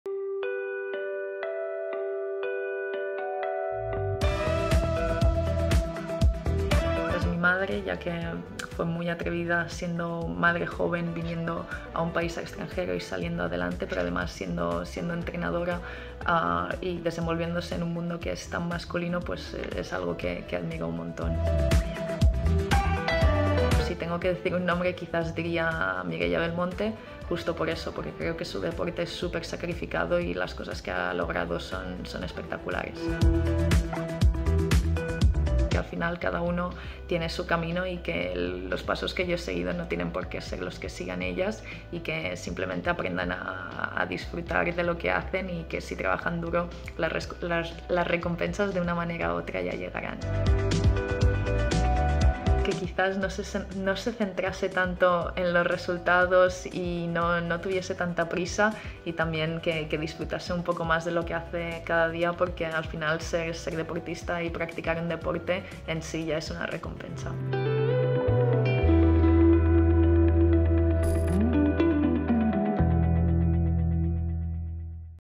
GALIA DVORAK: "Ser deportista ya es de por sí una recompensa"
La jugadora de tenis de mesa cuenta su historia, desvela que su máximo referente es su madre y que en el mundo olímpico su espejo es Mireia Belmonte.